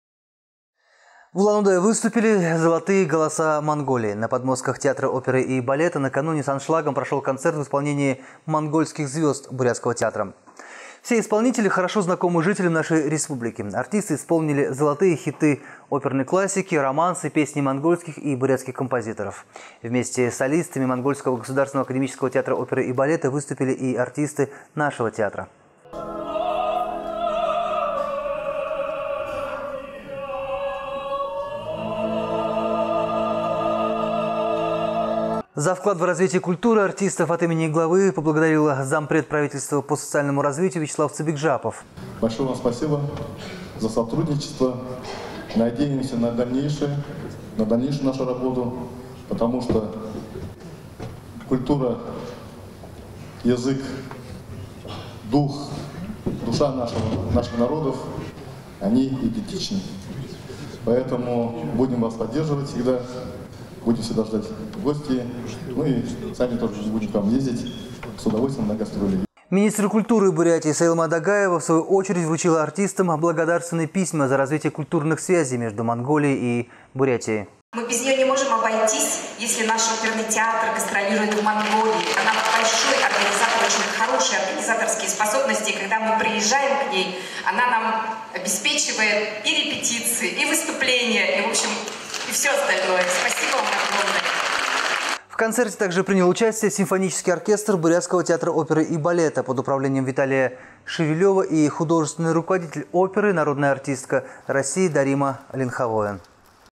В Улан-Удэ выступили «золотые голоса» Монголии
Улан-Удэ выступили «золотые голоса» Монголии. На подмостках Театра оперы и балета накануне с аншлагом прошел концерт в исполнении монгольских звёзд бурятского театра. Все исполнители хорошо знакомы жителям нашей республики. Артисты исполнили «золотые хиты» оперной классики, романсы, песни монгольских и бурятских композиторов. Вместе с солистами Монгольского государственного академического театра оперы и балета
выступили и артисты нашего театра.